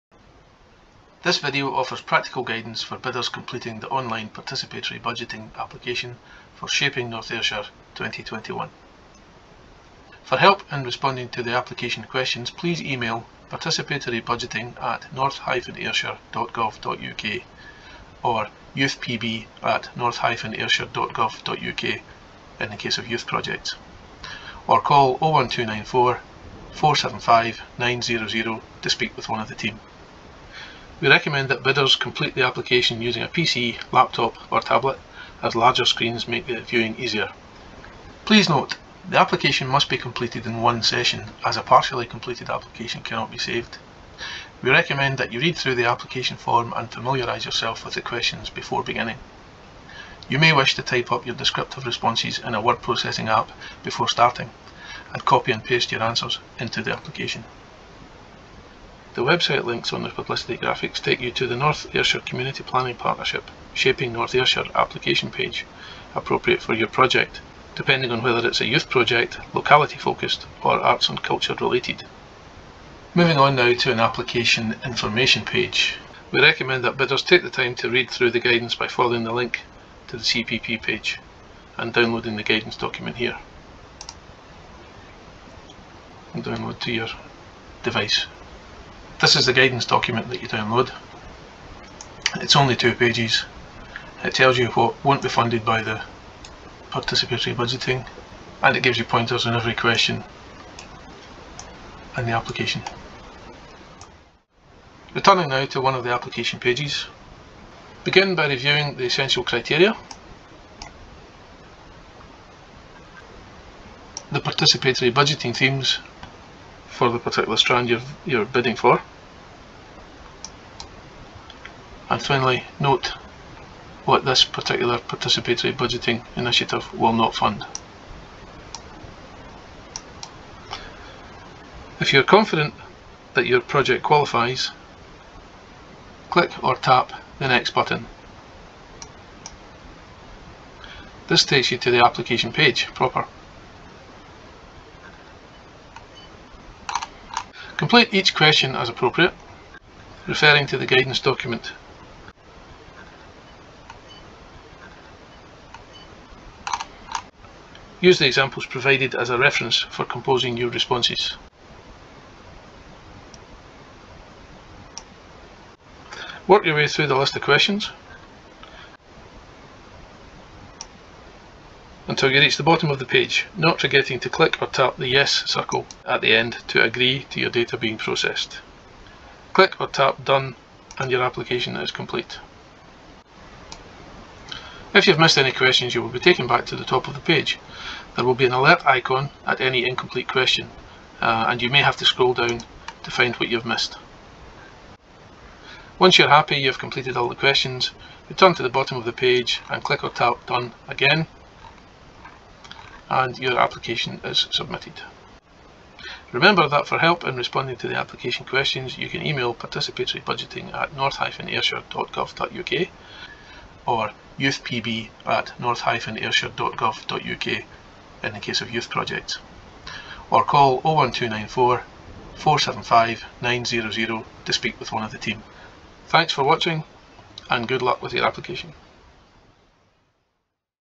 How to apply for Participatory Budgeting Funding in North Ayrshire [2021 Application Forms]
Local groups and individuals from across North Ayrshire's six localities (Arran, Garnock Valley, Irvine, Kilwinning, North Coast & Cumbraes and Three Towns) are invited to bid for participatory budgeting funding- for ideas and projects which will help make a difference in their communities.

Applications will be shortlisted and the outcomes decided by public voting early in 2021 when communities will have their say on the projects they believe could use funding to have the greatest impact.

Details and application forms can be found on the following links:
Youth Funding:

This useful video provides some pointers on completing your application.

Applications close 31st January at 5pm.